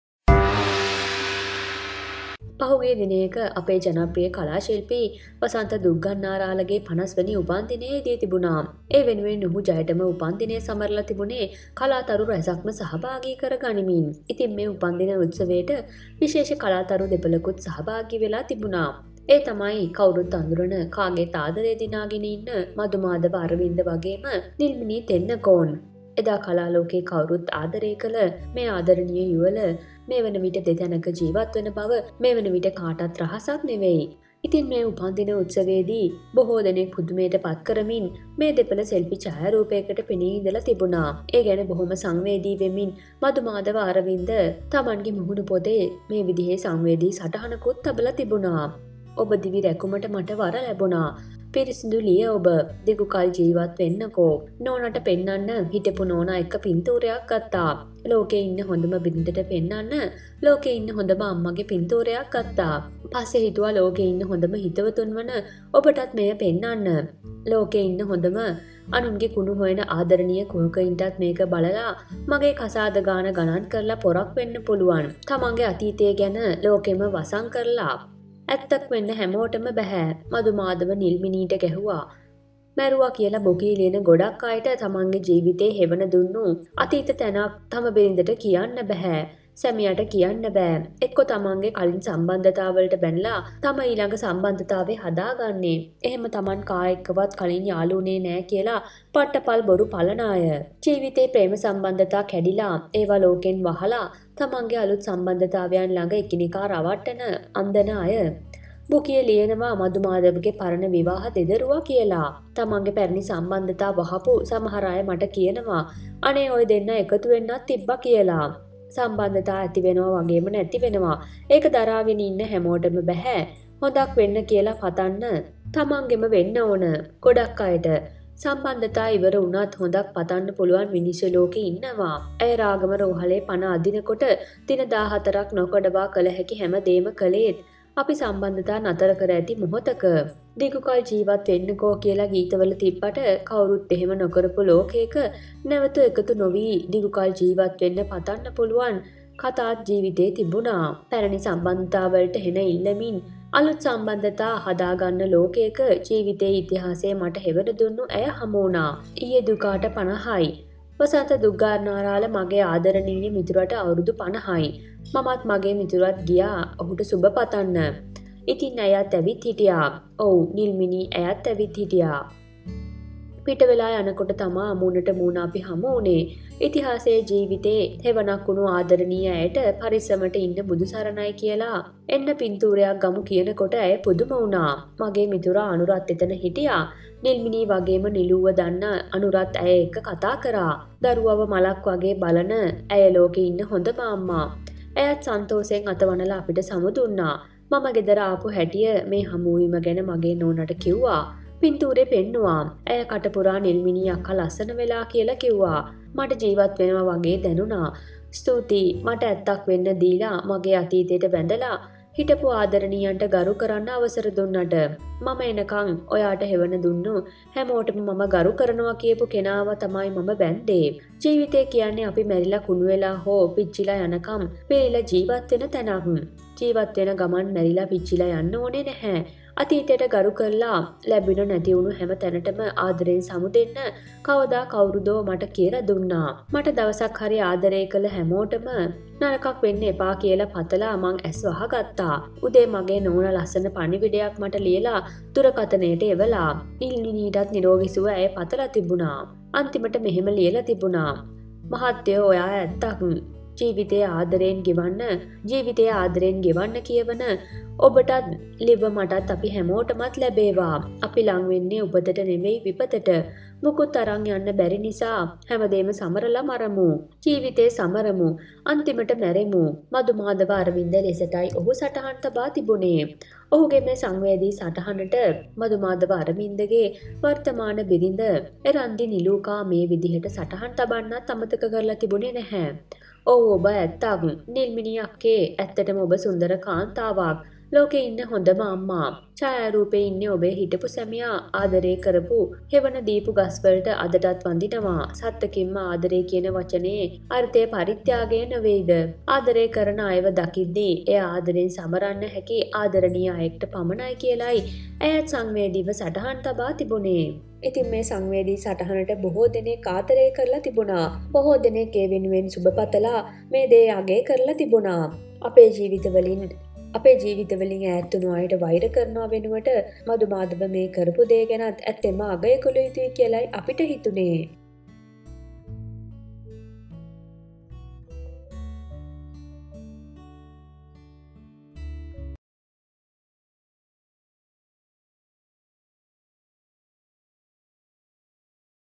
මධුමාධවයි නිල්මිණි තෙන්නකෝනුයි පාටි එකකදී මූනට මූන හමුවී සිදු වූ අපූරු සිදුවීම Nilmini Thennakoon
'ලෝකෙ ඉන්න හොදම බිරිදට පෙන්නන්න ලෝකෙ ඉන්න හොදම අම්මගේ ෆොටෝ එකක් ගත්තා
'  මධුමාධවයි නිල්මිණි තෙන්නකෝනුයි පාටි එකකදී මූනට මූන හමුවී සිදු වූ අපූරු සිදුවීම මෙන්න
 Madhumadhawa Arawinda and Nilmini Thennakoon